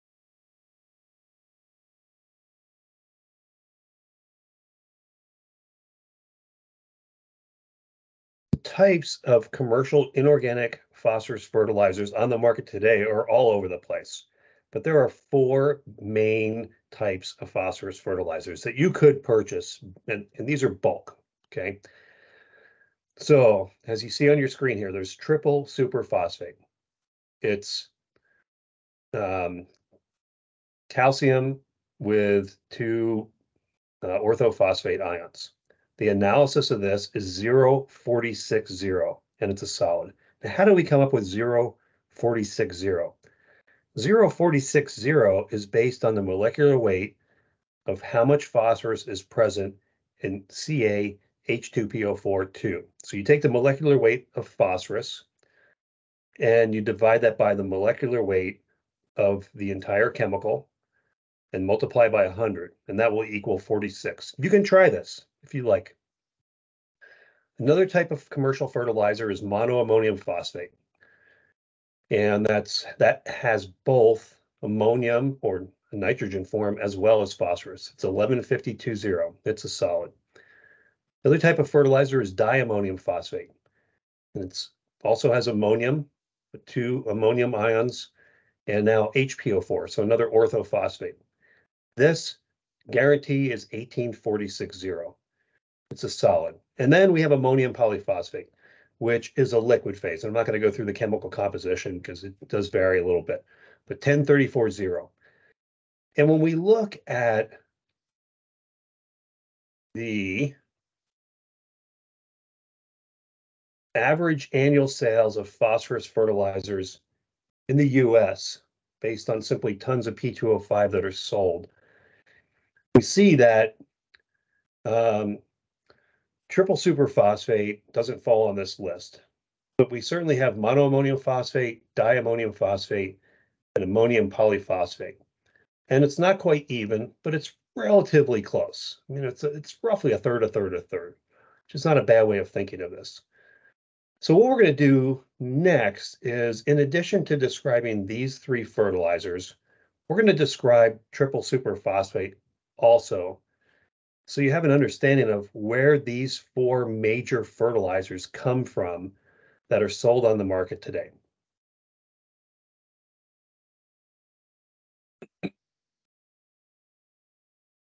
Week 9, Unit 6, Video 17 Introduction to Commercial P Fertilizers
This video provides an introduction to large scale commercial P fertilizers.